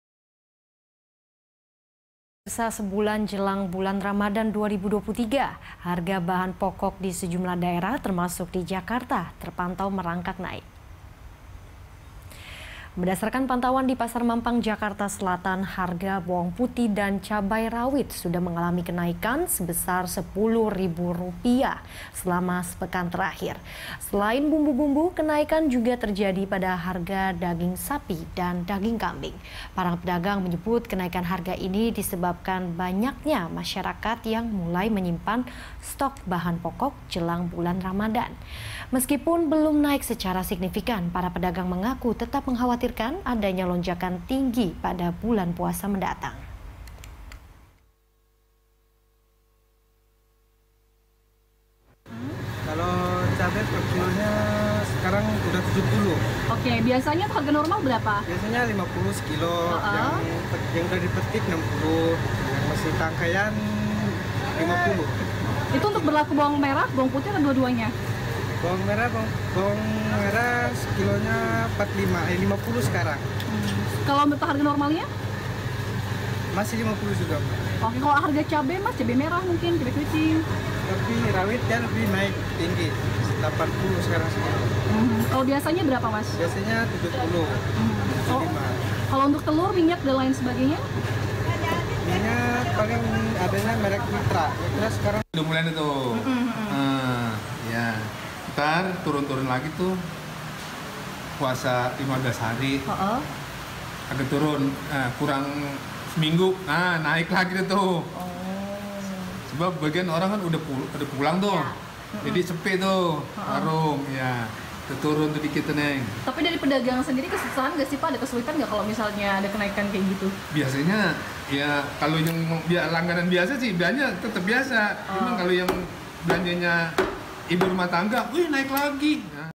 Harga Kebutuhan Pokok Merangkak Naik Jelang Ramadan
Sebulan jelang Ramadan, harga bahan pokok di sejumlah daerah termasuk Jakarta merangkak naik.

Saksikan program Beritasatu Siang setiap hari pukul 11.00 WIB di BTV, Bersatu Menginspirasi.